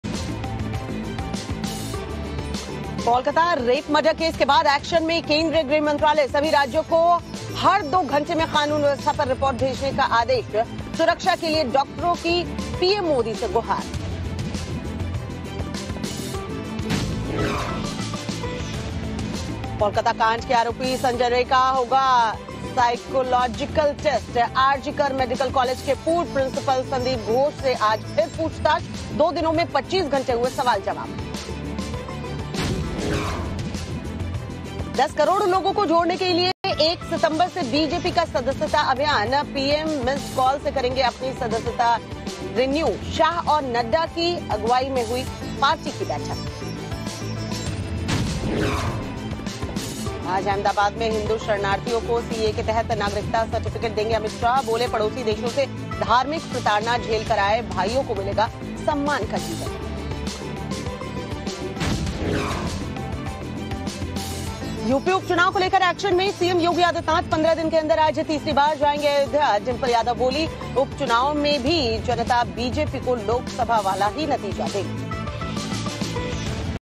सुबह की सबसे बड़ी खबरें | Top Morning Headlines Today | Kolkata Murder Case | Mamata Banerjee
Kolkata Murder Case News Updates | West Bengal के कोलकाता में R G Kar Medical Collage में लेडी डॉक्टर की हत्याकांड को लेकर हर रोज नए-नए खुलासे सामने आ रहे हैं। बता दें कि आरोपी संजय राय को गिरफ्तार कर लिया है। वहीं Doctors का देश के कई राज्यों में जबरदस्त प्रदर्शन जारी है। बता दें कि इस मामले को लेकर गृह मंत्री Amit Shah एक्शन में आ गए हैं। सभी राज्यों को कानूनी व्यवस्था पर रिपोर्ट भेजने के आदेश दिए हैं। सुरक्षा के लिए Doctors की PM Narendra Modi से गुहार। गौरतलब है कि R G Kar के पूर्व Principal, Sanjeev Ghosh से आज फिर पूछताछ होगी। अबतक 25 घंटे पूछताछ हुई। आरोपी संजय रॉय और शक के दायरे में आए लोगों को Phycological Test.